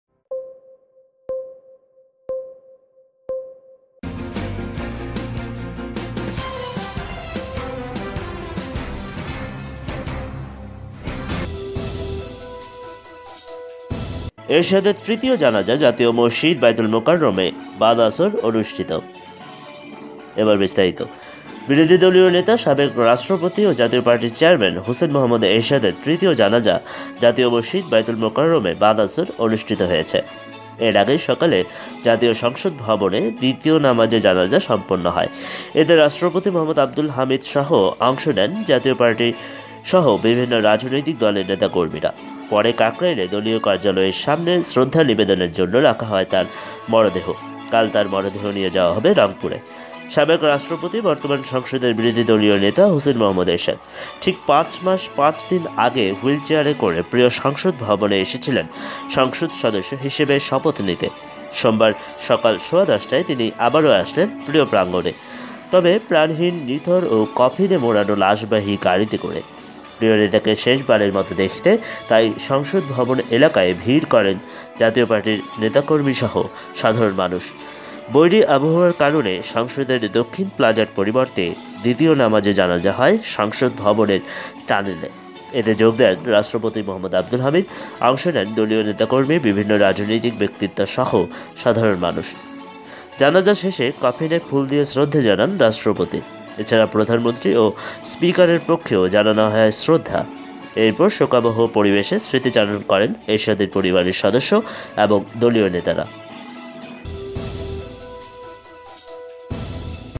এরশাদের তৃতীয় জানাজা জাতীয় মসজিদ বায়তুল মোকাররমে বাদ আসর অনুষ্ঠিত
বিরোধীদলীয় নেতা, সাবেক রাষ্ট্রপতি ও জাতীয় পার্টির চেয়ারম্যান হুসেইন মুহম্মদ এরশাদের তৃতীয় জানাজা জাতীয় মসজিদ বায়তুল মোকাররমে বাদ আসর অনুষ্ঠিত হয়েছে। এর আগে সকালে জাতীয় সংসদ ভবনে ২য় নামাজে জানাজা সম্পন্ন হয়। এতে রাষ্ট্রপতি মুহম্মদ আব্দুল হামিদসহ অংশ নেন জাতীয় পার্টিসহ বিভিন্ন রাজনৈতিক দলের নেতা-কর্মীরা। পরে কাকরাইলে দলীয় কার্যালয়ের সামনে শ্রদ্ধা নিবেদনের জন্য রাখা হয় তার মরদেহ। কাল তার মরদেহ নিয়ে যাওয়া হবে রংপুরে।

সাবেক রাষ্ট্রপতি, বর্তমান সংসদের বিরোধী দলীয় নেতা হুসেইন মুহম্মদ এরশাদ। ঠিক ৫ মাস ৫ দিন আগে হুইল চেয়ারে করে প্রিয় সংসদ ভবনে এসেছিলেন সংসদ সদস্য হিসেবে শপথ নিতে। সোমবার (১৫ জুলাই) সকাল সোয়া ১০টায় তিনি আবারো আসলেন প্রিয় প্রাঙ্গণে। তবে, প্রাণহীন, নিথর ও কফিনে মোড়ানো লাশবাহী গাড়িতে করে। প্রিয় নেতাকে শেষ বারের মতো দেখতে তাই সংসদ ভবন এলাকায় ভিড় করেন জাতীয় পার্টির নেতাকর্মীসহ সাধারণ মানুষ।

বৈরী আবহাওয়ার কারণে সংসদের দক্ষিণ প্লাজার পরিবর্তে ২য় নামাজে জানাজা হয় সংসদ ভবনের টানেলে। এতে যোগ দেন রাষ্ট্রপতি মো.আবদুল হামিদ। অংশ নেন দলীয় নেতাকর্মী, বিভিন্ন রাজনৈতিক ব্যক্তিত্বসহ সাধারণ মানুষ।

জানাজা শেষে কফিনে ফুল দিয়ে শ্রদ্ধা জানান রাষ্ট্রপতি। এছাড়া প্রধানমন্ত্রী ও স্পিকারের পক্ষেও জানানো হয় শ্রদ্ধা। এরপর শোকাবহ পরিবেশে স্মৃতি চারণ করেন এরশাদের পরিবারের সদস্য এবং দলীয় নেতারা।

এরশাদের স্ত্রী ও জাতীয় পার্টির সিনিয়র কো-চেয়ারম্যান রওশন এরশাদ বলেন, জনগণের জন্য ভালো একটি জীবন দিতে চেষ্টা করেছেন। কতটুকু করতে পেরেছে তা এখন জনগণই বিচার করবেন। জানাজায় অংশ নেয়া রাজনৈতিক নেতারা ব্যক্তি এরশাদকে অমায়িক ব্যবহারের একজন মানুষ হিসেবে অভিহিত করে তাঁর রাজনৈতিক জীবনের মূল্যায়ন জনগণের করবে বলে মন্তব্য করেছেন।

তোফায়েল আহমেদ বলেন, এরশাদ ব্যক্তিগত জীবনে ওমাইয়িক ব্যবহারের একজন মানুষ ছিলেন।

পরে, সোমবার সকাল সাড়ে ১১টায় দলীয় নেতা-কর্মীসহ সর্বসাধারণের শ্রদ্ধা নিবেদনের জন্য হুসেইন মুহম্মদ এরশাদের মরদেহ নিয়ে যাওয়া হয় রাজধানীর কাকরাইলে জাতীয় পার্টির কেন্দ্রীয় কার্যালয়ে। সেখানে প্রিয় নেতাকে শেষ বারের মত দেখতে ভিড় জমান জাতীয় পার্টির নেতা-কর্মীরা।

জাতীয় পার্টির প্রেসিডিয়াম সদস্য রুহুল আমিন হাওলাদার বলেন, দেশের জন্য দেশের মানুষের জন্য তিনি একজন নিবেদিত মানুষ ছিলেন। কাকরাইল থেকে এরশাদের মরদেহ নিয়ে যাওয়া হয় বায়তুল মোকাররম জাতীয় মসজিদে। সেখানে বাদ আসর তার তৃতীয় নামাজে জানাজা অনুষ্ঠিত হয়।

মঙ্গলবার সকালে হেলিকপ্টারে করে এরশাদের মরদেহ তার পৈতৃক নিবাস রংপুরে নিয়ে যাওয়া হবে। সেখানে ৪র্থ ও সবশেষ জানাজা অনুষ্ঠিত হওয়ার কথা রয়েছে। তবে মঙ্গলবার তার দাফনের কথা থাকলেও কোথায় করা হবে সে বিষয়ে দল বা পরিবার থেকে চূড়ান্ত সিদ্ধান্ত এখনো জানা যায়নি।

৯০ বছর বয়সী হুসেইন মুহম্মদ এরশাদ ঢাকার সম্মিলিত সামরিক হাসপাতালে ১০ দিন লাইফ সাপোর্টে থাকার রোববার সকাল ৭টা ৪৫ মিনিটে জীবনের মায়া ছেড়ে চলে যান।